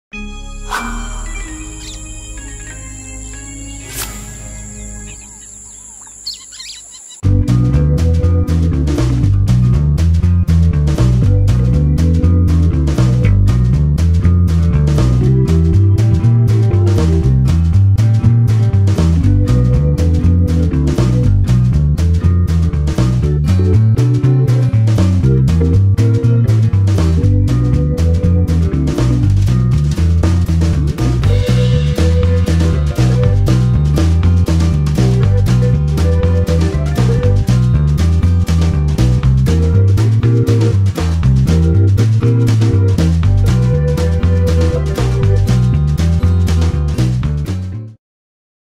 El crucero Celebrity Equinox zarpa desde el puerto de Alicante
Time Lapse - Celebrity Equinox cruise sets from Alicante's harbour.